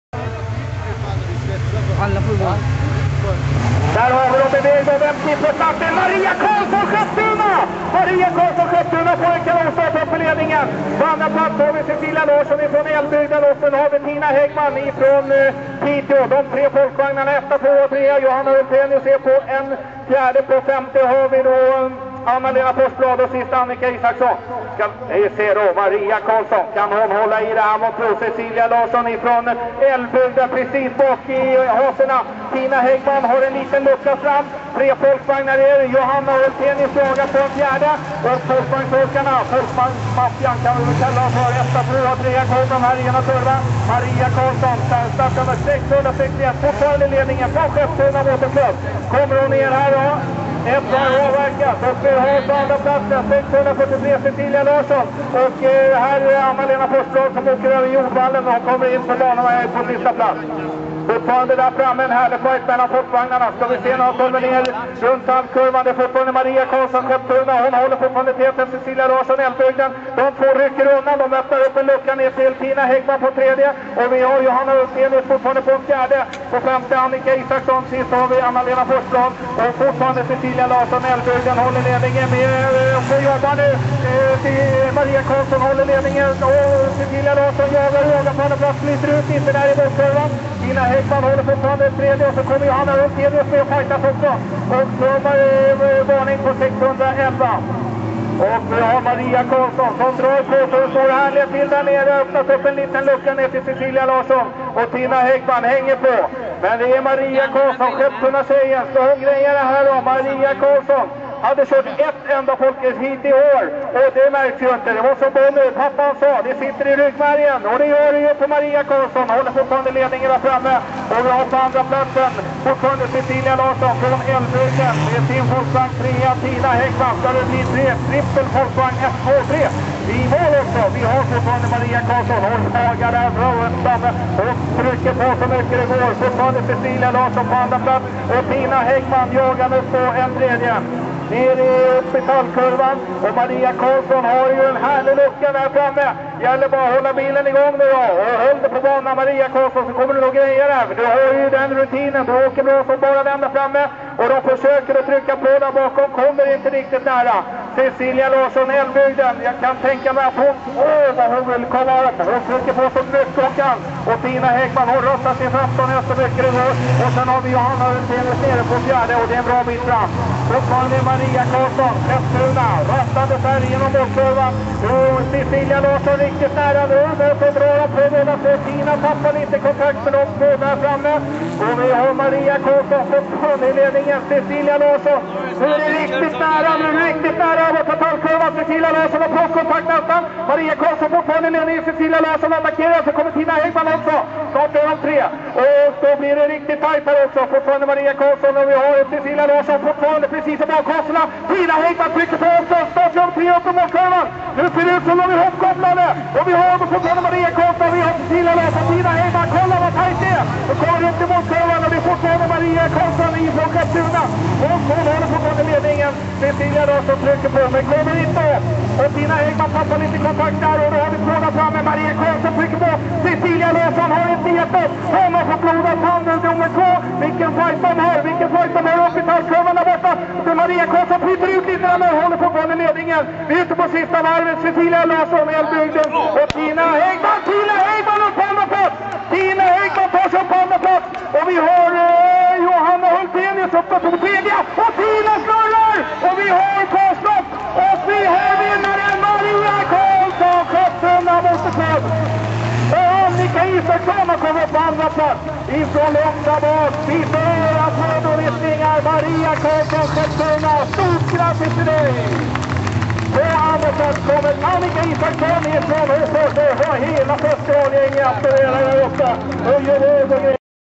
Semesterracet 09 A final Dam
A-finalen Damer där Jönköping MKs Johanna hultenius blir 3a och Anna-lena Forsblad 5a